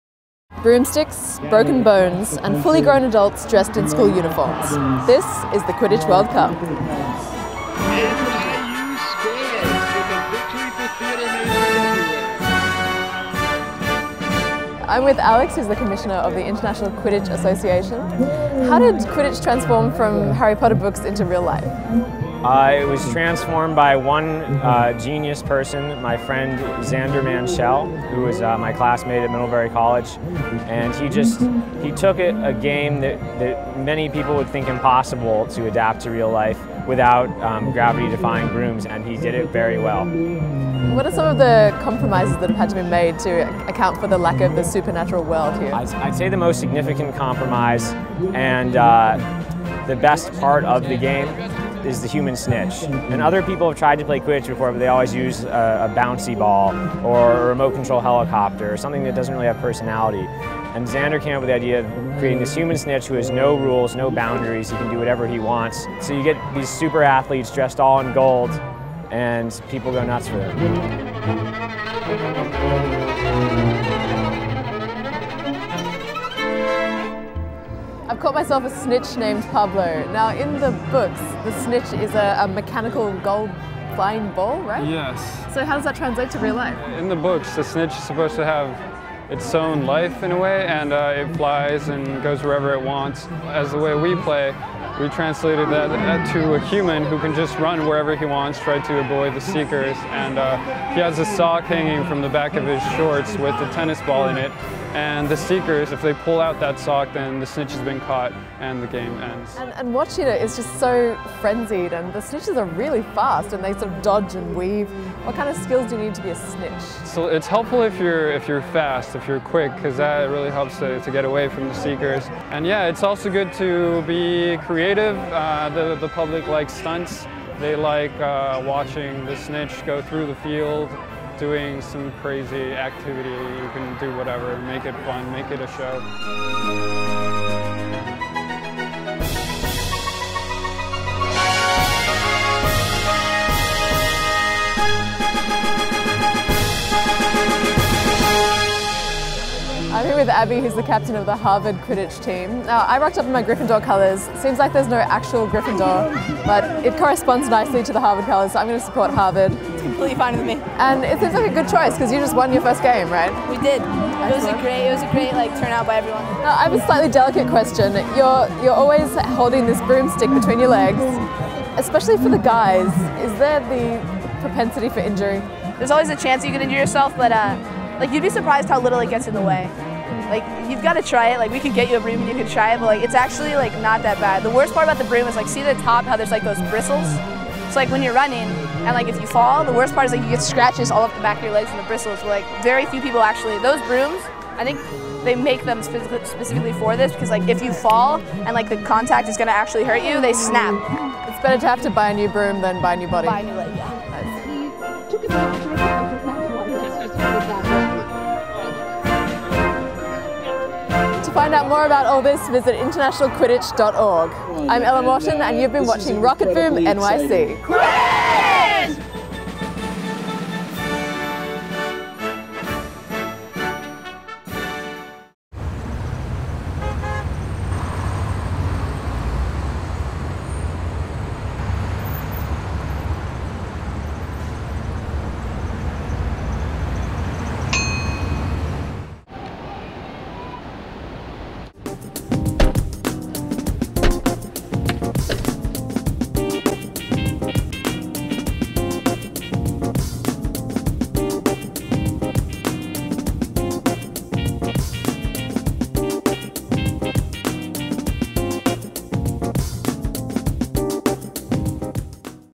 The Quidditch World Cup
Click on the link above for more info on today's episode!

Inspired by the Harry Potter books, players and organizers of the game of Quidditch have turned a fantasy sport into reality!